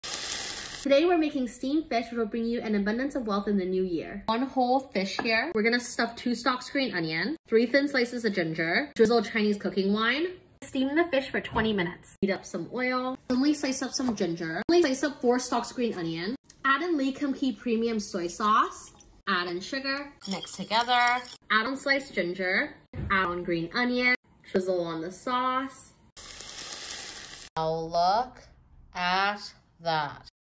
Chinese Steamed Fish (Easy & Authentic)
Steamed Fish is a must-have during Lunar New Year; it brings an abundance of wealth in the new year. A famous saying "年年有鱼" in Chinese is a play on words that means by having fish; you will have surplus overflowing every year! For extra luck, make sure always to have the head of the fish facing the eldest person in the room.